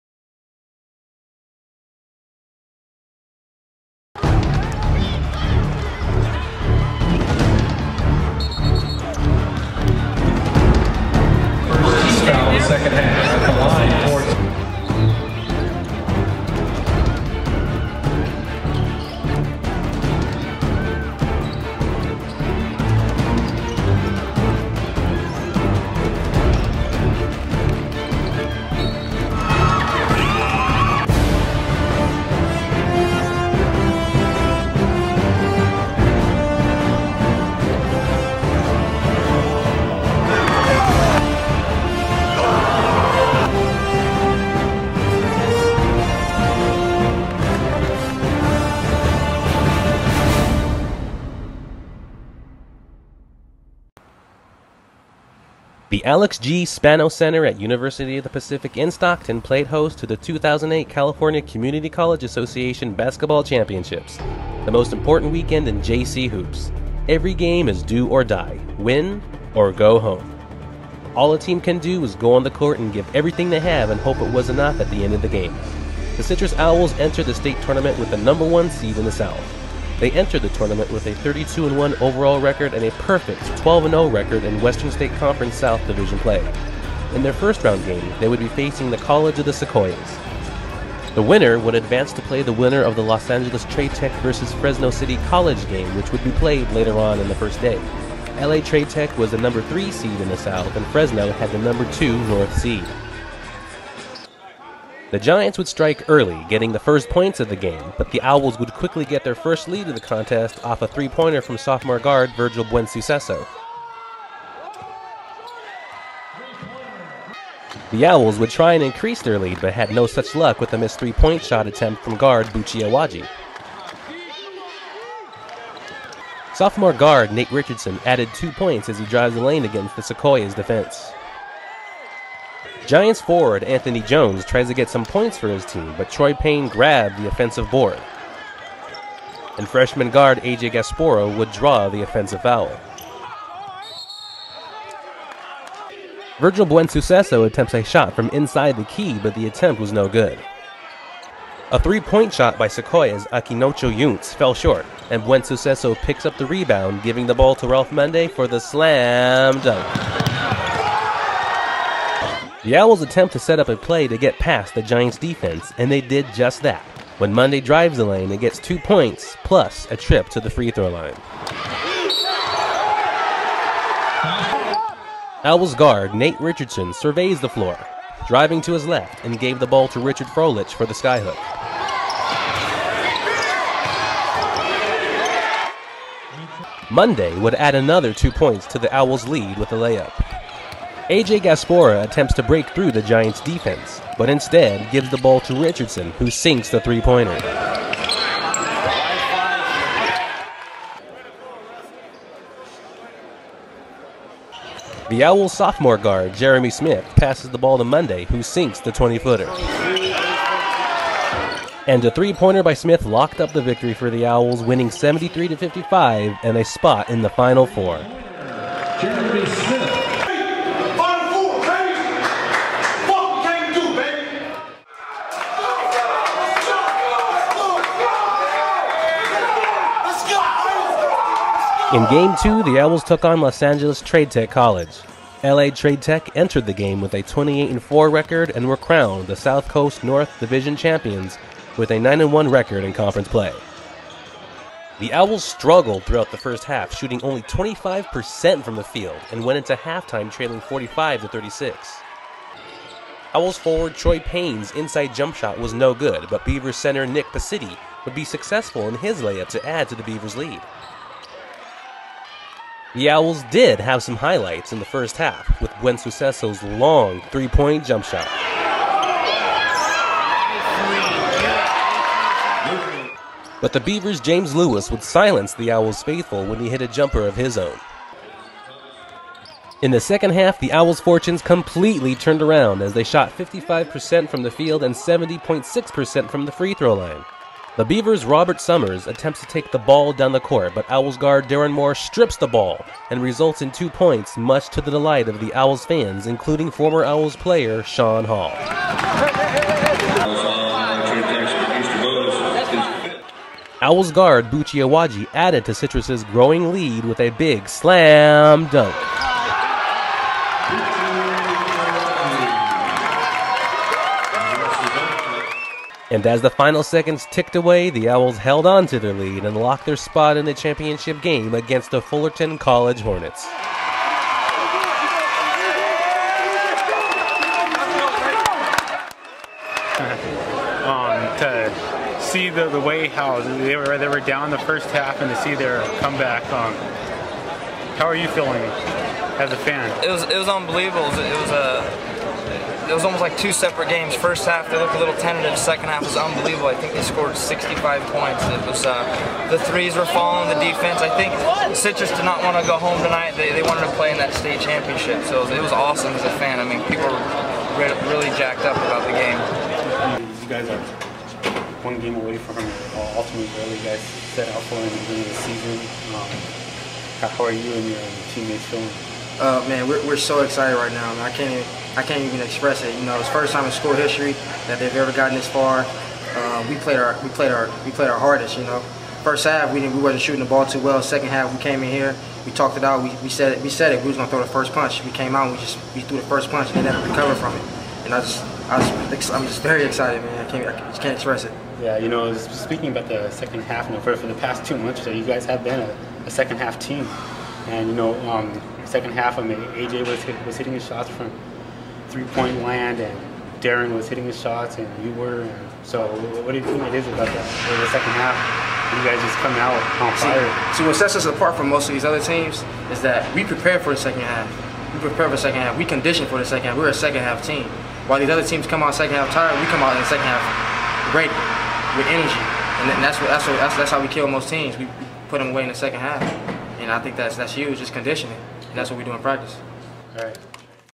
Citrus College Men's Basketball Championship, 2008: pt. 1
So I shot this during my time on staff at Citrus College's newspaper, the Clarion newspaper. This was an amazing championship run. This video is also available on the Clarion's I am reposting them because the originals audio track is unsynchronized from the video. Enjoy!